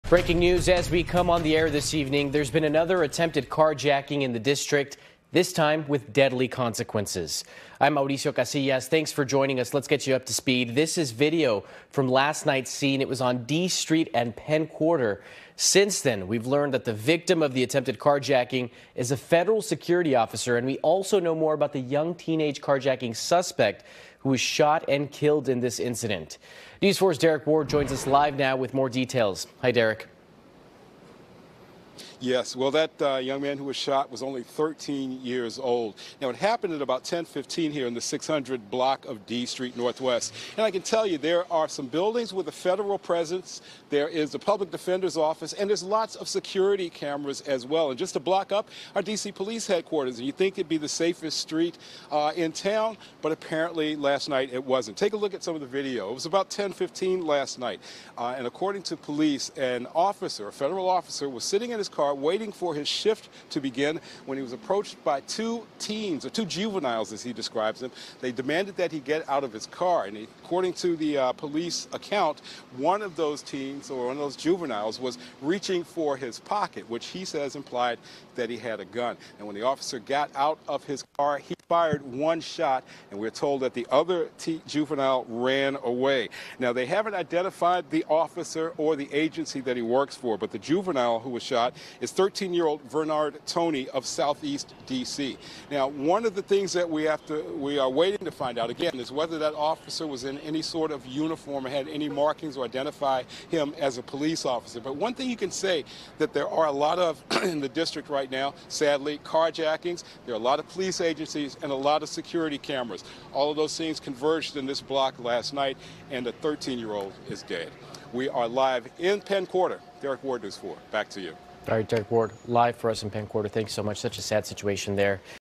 Security officer kills 13-year-old carjacking suspect in Penn Quarter | NBC4 Washington
A 13-year-old boy has died after he was shot during a carjacking in Northwest D.C. on Saturday, according to D.C. police. The victim of the carjacking was described as an off-duty federal security officer. News4’s Derrick Ward reports.